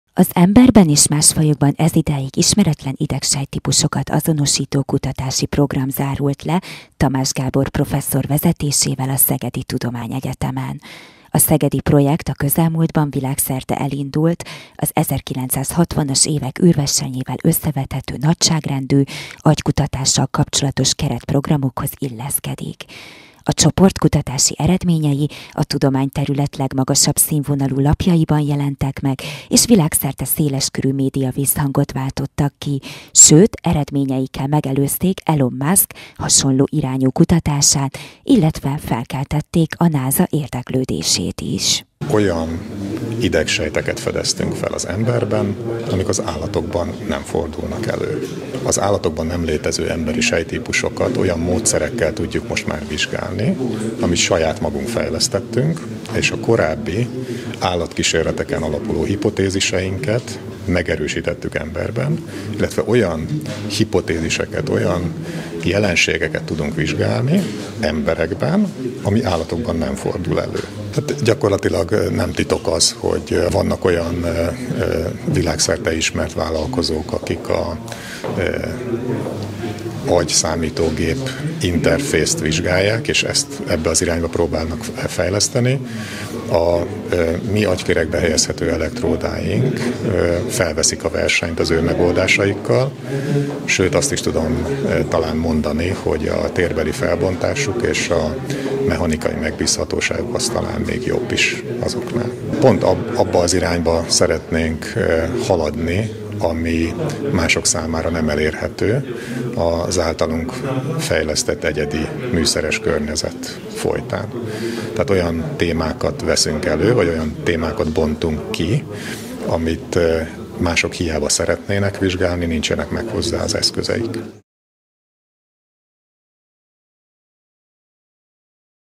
Ezidáig ismeretlen idegsejttípusokat fedeztek fel az SZTE-n
Az emberben és más fajokban ezidáig ismeretlen idegsejttípusokat azonosító kutatási program zárult le Tamás Gábor professzor vezetésével a Szegedi Tudományegyetemen. A szegedi projekt a közelmúltban világszerte elindult - az 1960-as évek űrversenyével összevethető nagyságrendű - agykutatással kapcsolatos keretprogramokhoz illeszkedik.

Operatőr: Magyar Andor